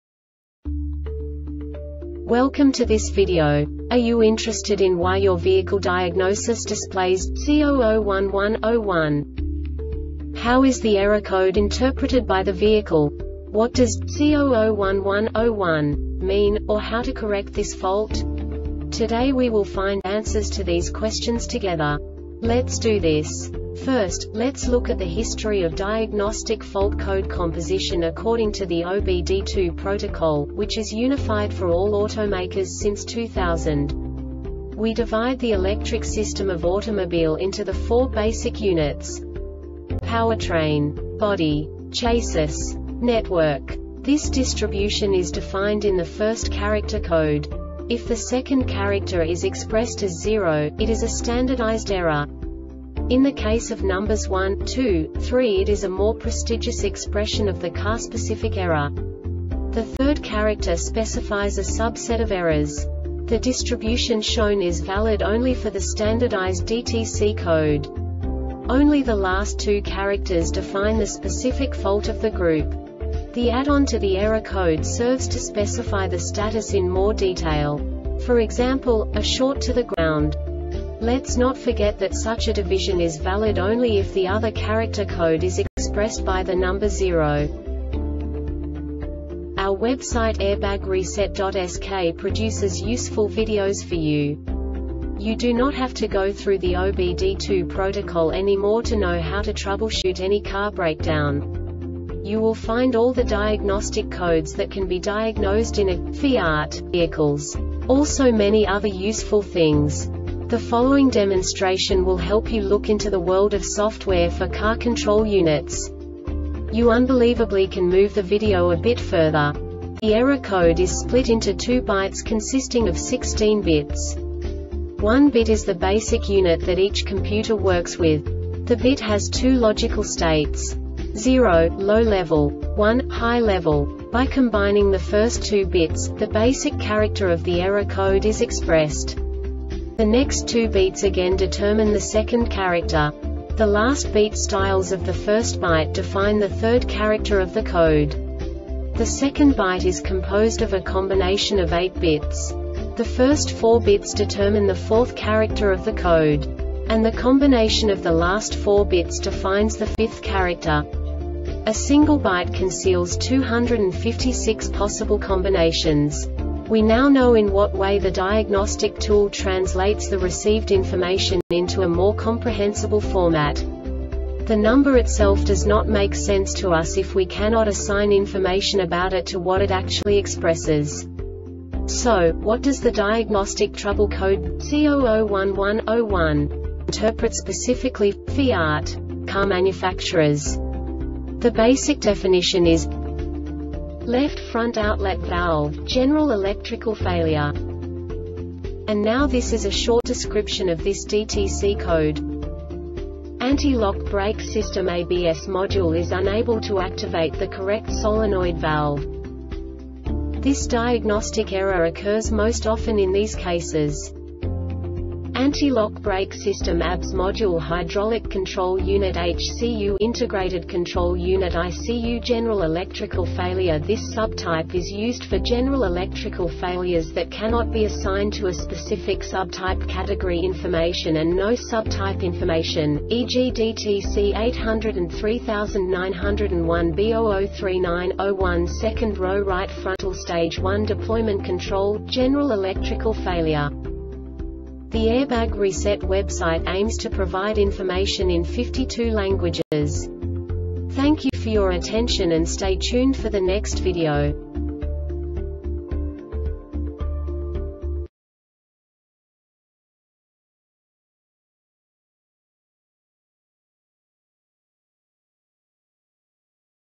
DTC Fiat C0011-01 Short Explanation
Contents:
0:21 Basic DTC analysis according to OBD2 protocol standard.
1:48 Insight into programming
2:58 A diagnostically understandable description of the Diagnostic Trouble Code
3:05 Basic error definition

Make:
FIAT
Code:
C0011-01
Definition:
Left Front Outlet Valve - General Electrical Failure
Description:
Anti-lock Brake System (ABS) Module is unable to activate the correct solenoid valve.
Cause:
Anti-Lock Brake System (Abs) Module
Hydraulic Control Unit (Hcu)
Integrated Control Unit (Icu)
Failure Type: